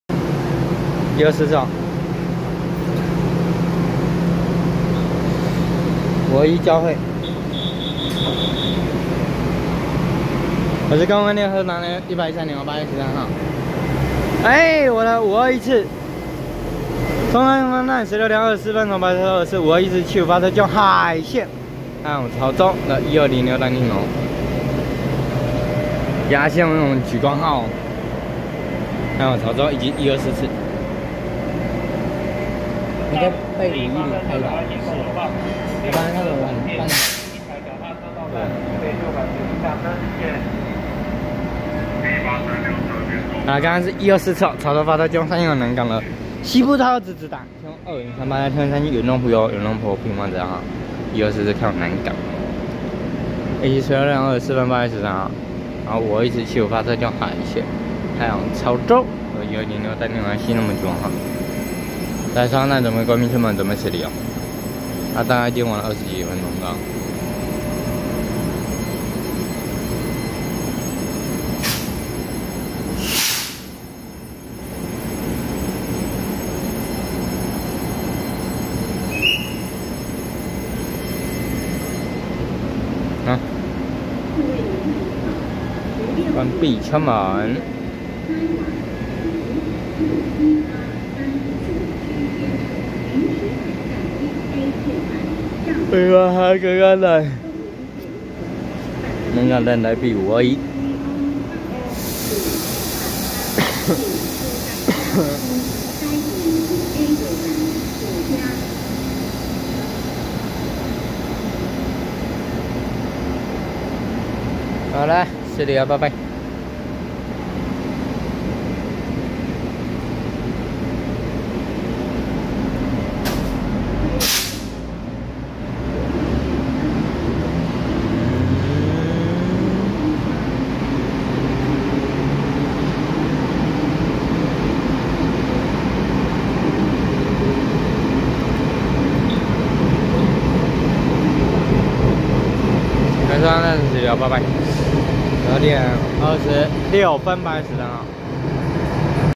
124次潮州發車經由山線開往南港的西部超優質直達TEMU2038+TEMU2037原裝普悠猴原裝普猴普悠瑪自強號，521次七堵發車經由海線開往潮州的E206單電頭+新式自動門莒光號
113/8/13松山站16:24分拍攝124次潮州發車經由山線開往南港的西部超優質直達TEMU2038+TEMU2037原裝普悠猴原裝普猴普悠瑪自強號，521次七堵發車經由海線開往潮州的E206單電頭+新式自動門莒光號